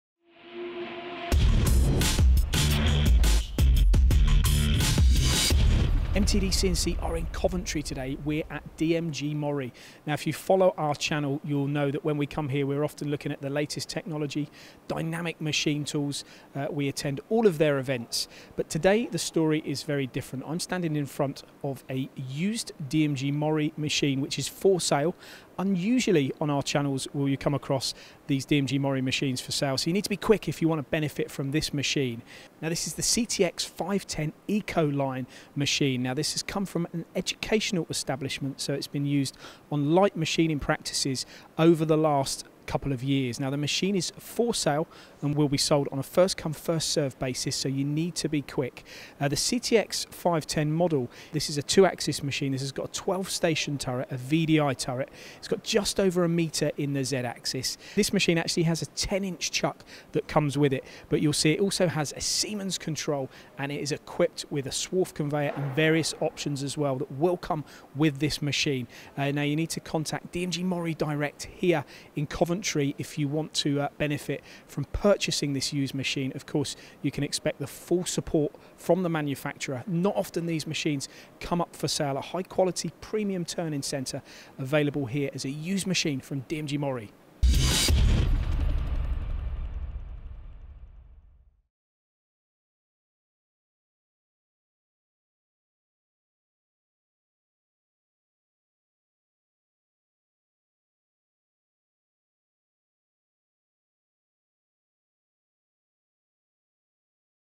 DMG MORI CTX 510 ecoline for sale as new
MTDCNC visited DMG MORI this week and for one lucky engineering company they have a nearly new DMG MORI CTX 510 available and ready to deliver immediately  The benefits of this machine is 2 axis, 12 station turret, 10 inch chuck, Siemens control and a swarf conveyor.